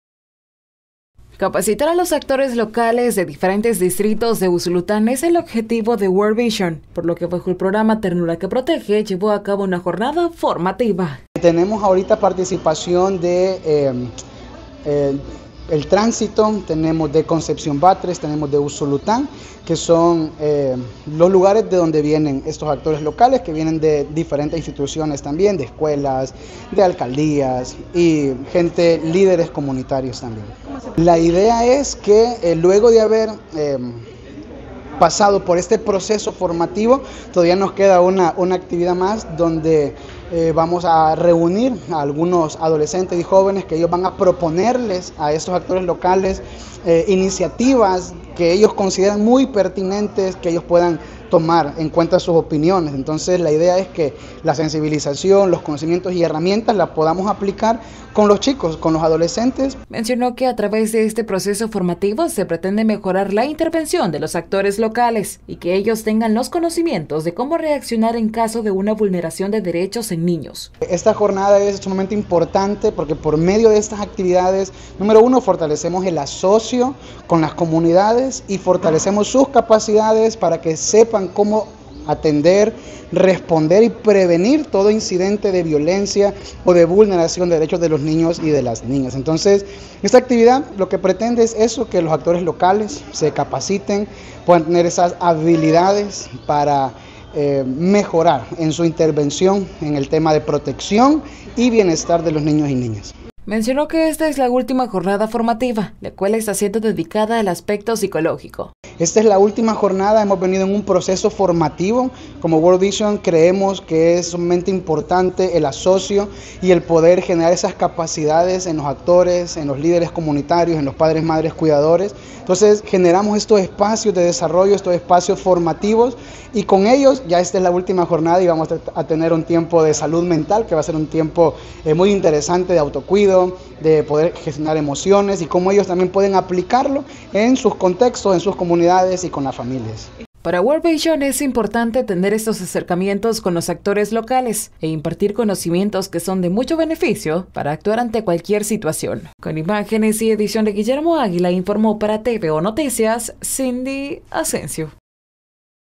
World visión llevó acabo una jornada formativa con actores locales de los distritos de Usulután
Bajo el programa ternura que protege World visión llevó acabo una jornada formativa con actores locales de diferentes distritos de Usulután Este, con el objetivo de dar a conocer la forma de reaccionar en caso de una vulneración de derechos en los niños.